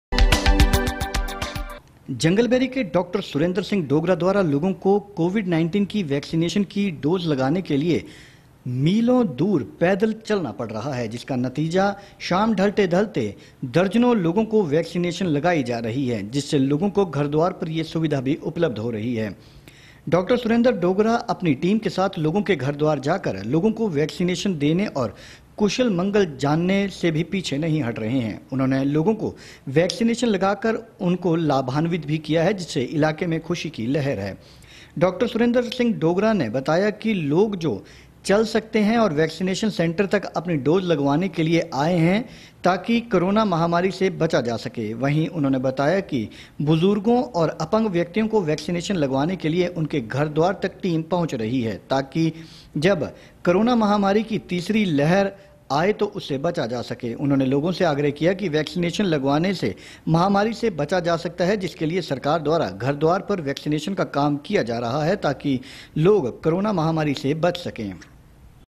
SUJANPURडॉ सुरेंद्र सिंह डोगरा द्वारा मिलो चलकर लगाया वैक्सीनेशन ग्रामीणों ने किया डॉक्टर का धन्यवाद
जंगल बेरी के डॉक्टर सुरेंद्र सिंह डोगरा द्वारा लोगों को कोविड-19 की वैक्सीनेशन की  डोज लगाने के लिए मिलो दूर पैदल चलना पड़ा जिसका नतीजा शाम ढलते ढलते दर्जनों लोगों को वैक्सीनेशन लगा दी,जिससे लोगों को सुविधा मिली है जिसमें उन्होंने अपनी टीम के साथ लोगों के घर द्वार जाकर लोगों को  वैक्सीनेशन देने और कुशल मंगल जाना, उन्होंने लोगों की वैक्सीनेशन लगाकर उनको लाभान्वित किया है जिससे इलाके में खुशी की लहर दौड़ हुई है वही सुरेंद्र सिंह डोगरा ने बताया कि लोग जो चल सकते हैं वैक्सीनेशन सेंटर तक अपनी डोज लगवाने के लिए आएं ताकि करो ना महामारी से बचा जा सके, वहीं उन्होंने बताया कि बुजुर्गों और अपंग व्यक्तियों को वैक्सीनेशन लगवाने के लिए उनके घर द्वार तक टीम में पहुंचेंगे ताकि जब करो ना महामारी की तीसरी लहर   से बचा जा सके, उन्होंने लोगों से आग्रह किया है की वैक्सीनेशन लगाने से महामारी से बचा जा सकता है जिसके लिए सरकार द्वारा घर द्वार वैक्सीनेशन का कार्य भी किया जा रहा है ताकि लोग करो ना महामारी से बचा जा सके